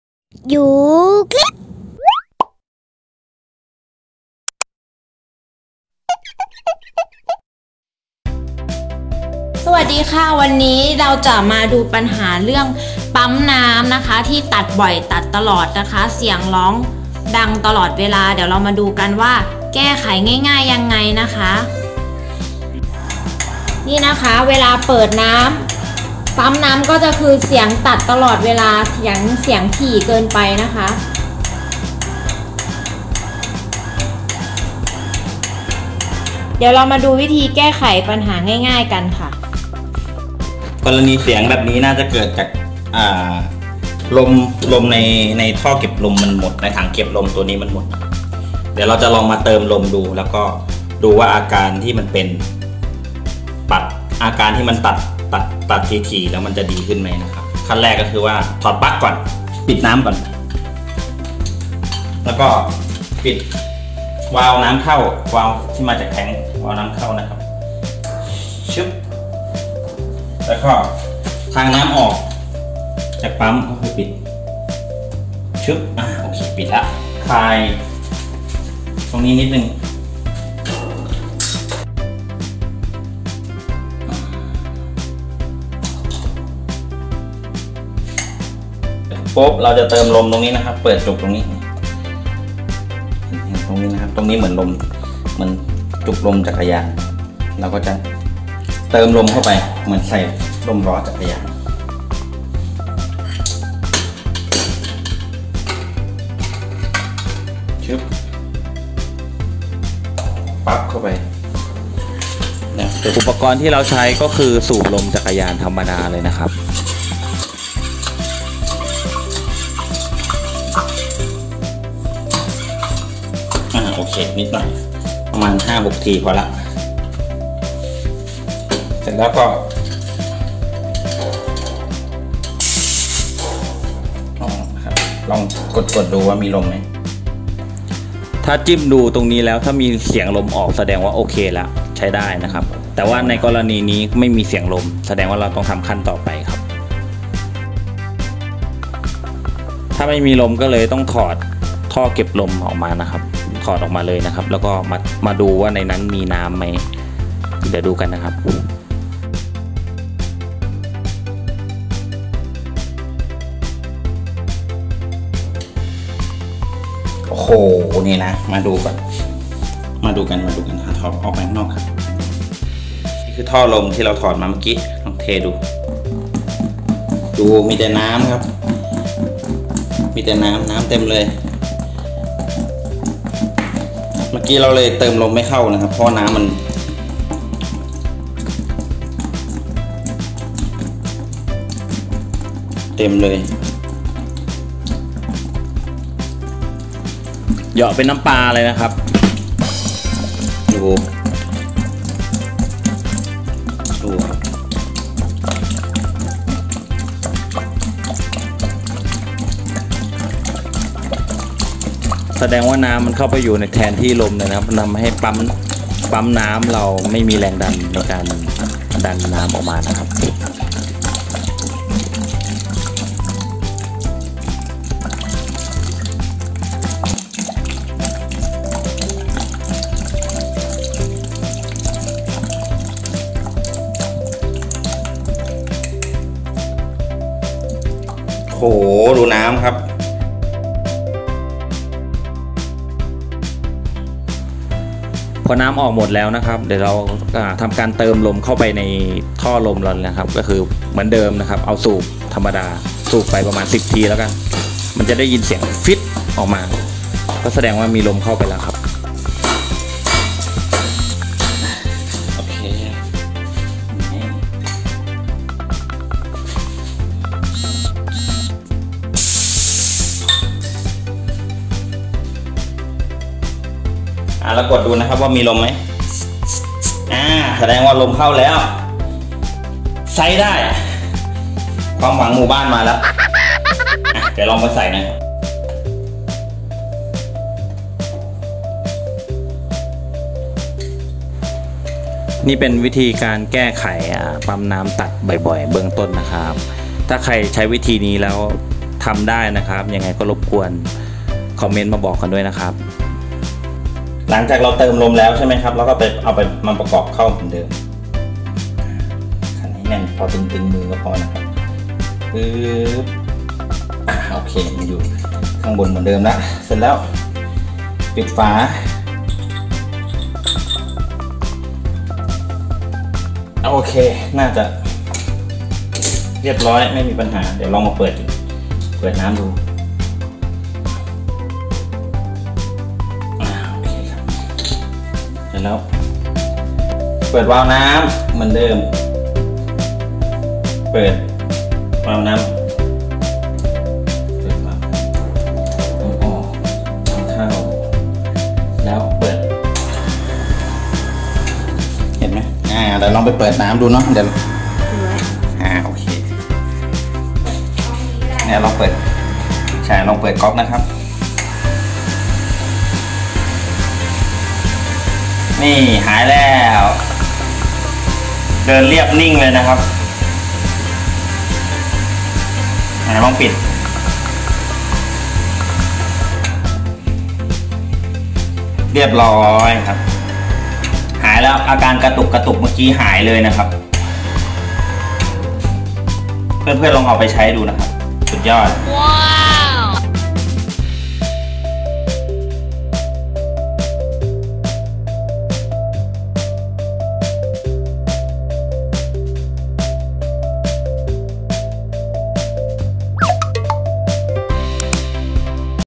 วิธีแก้ไข ปั๊มน้ำตัดถี่ ปั๊มน้ำกระตุก ปั๊มน้ำตัดบ่อย ปั๊มน้ำเสีย ซ่อมปั๊มน้ำง่ายๆ ด้วยตัวเอง|Yu clip
คลิปนี้เรามีเทคนิคแก้ไขปั๊มน้ำตัดถี่ๆ ตัดบ่อย เปิดน้ำทีไร มีเสียงตัดรัวๆตลอดเวลา ด้วยวิธีง่ายๆ ใช่แค่ที่สูบลมจักรยานเท่านั้นเอง

ง่ายมากๆ ผู้หญิงก็ทำได้เองนะ

ลองซ่อมกันดู รับรองได้ผล 100%

หากเพื่อนๆคนไหนลองทำแล้วได้ผล และเห็นว่าคลิปนี้มีประโยชน์ ฝากแชร์ไปให้ความรู้กับเพื่อนคนอื่นๆ ก่อนปั๊มน้ำจะไหม้ไปซะก่อน แล้วคอมเม้นท์มาคุยกันนะ

ขอบคุณทุกคนที่ติดตามช่องเล็กๆของเราค่ะ
หวังว่าทุกคนจะชอบวิดิโอของเรา และสามารถติดตาม พูดคุยกับเราได้ที่ช่องทางเหล่านี้ค่ะ